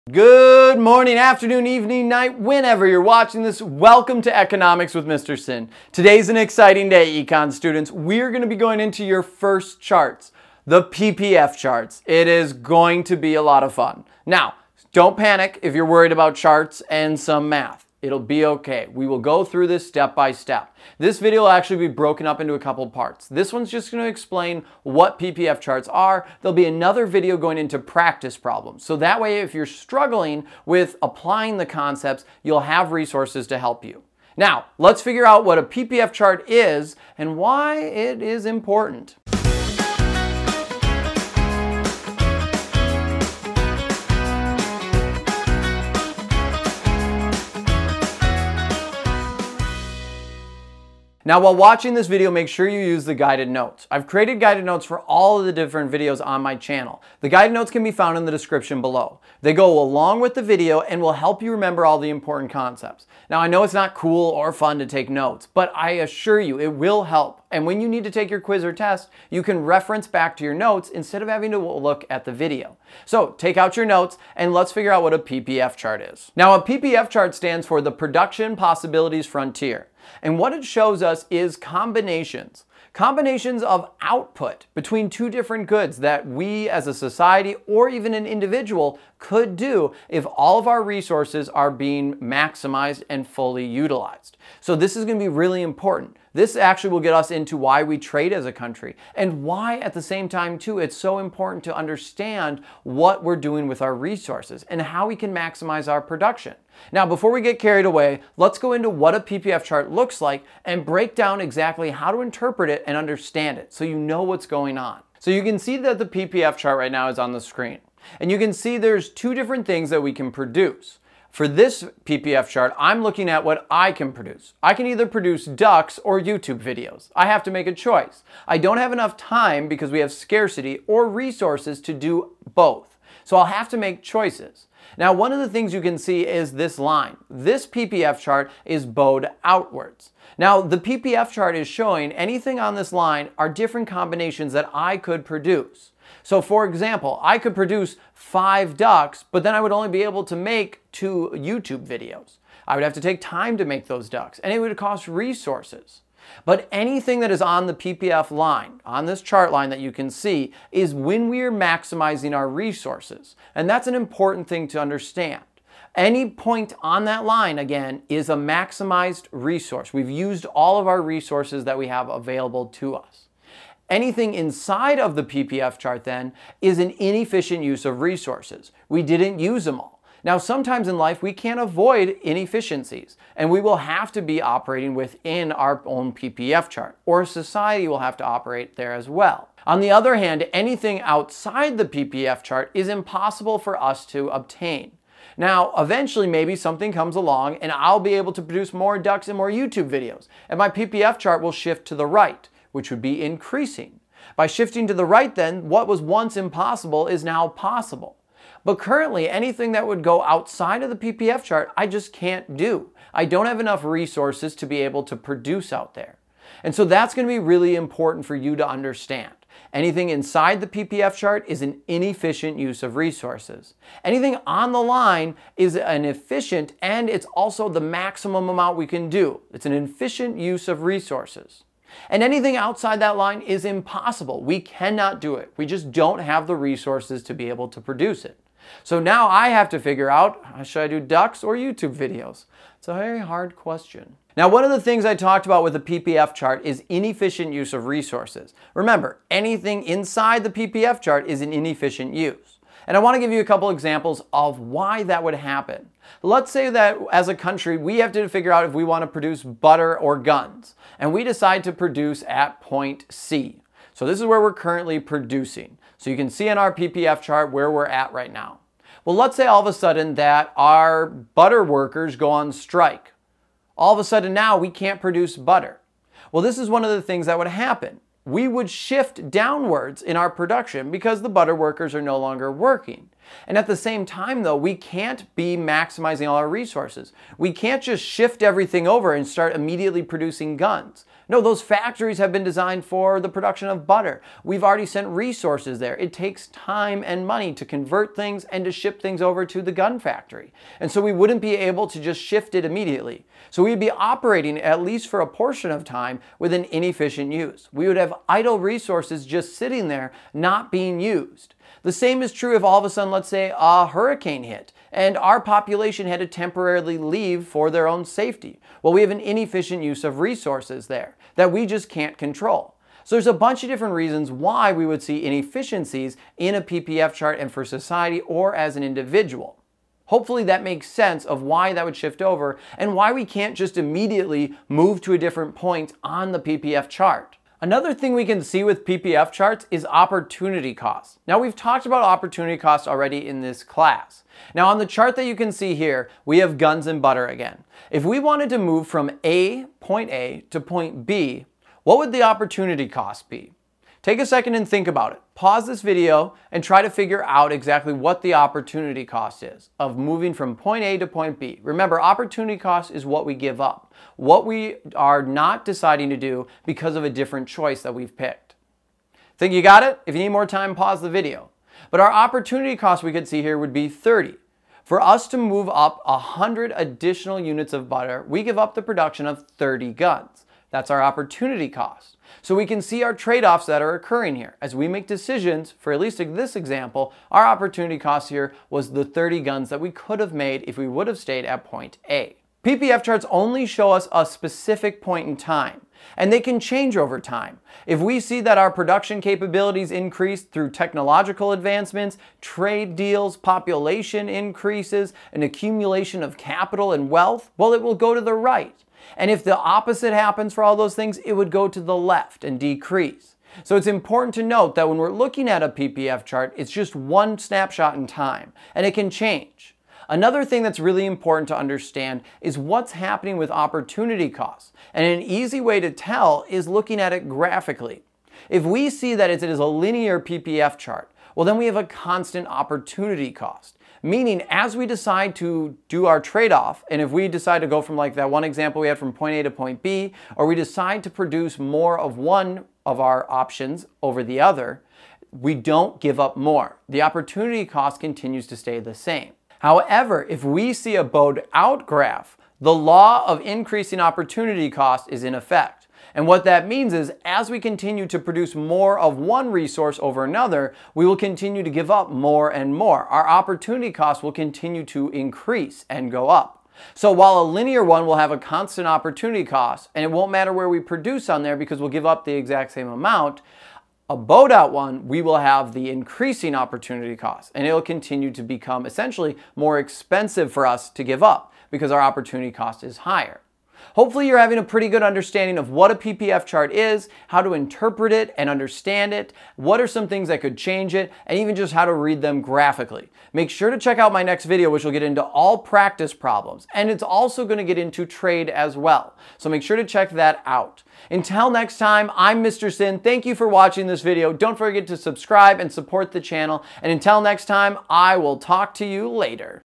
Production Possibilities Frontier: Everything you need to know!
This video goes over PPF charts. It explains how to read them and how they show different output levels in society.